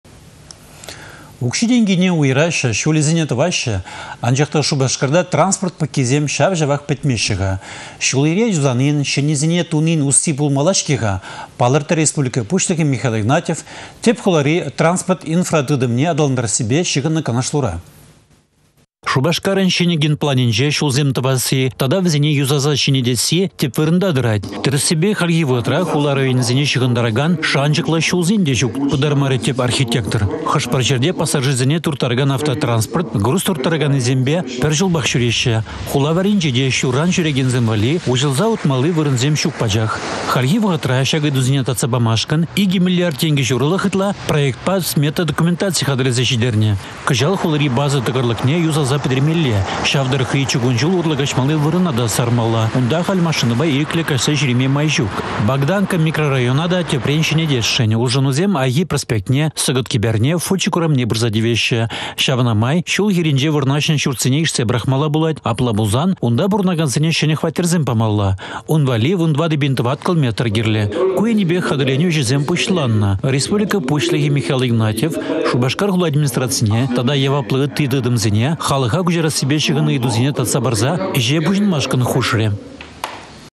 Шупашкарăн транспорт инфраструктурине çĕнетмелле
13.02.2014
Укçи-тенкине уйăраççĕ, çулĕсене тăваççĕ, анчах та Шупашкарта транспорт пăккисем çав-çавах пĕтмеççĕ. Çул-йĕре юсанин, çĕннисене тунин усси пулмалла-çке-ха — палăртрĕ Республика пуçлăхĕ Михаил Игнатьев тĕп хулари транспорт инфратытăмне аталантарассипе çыхăннă канашлура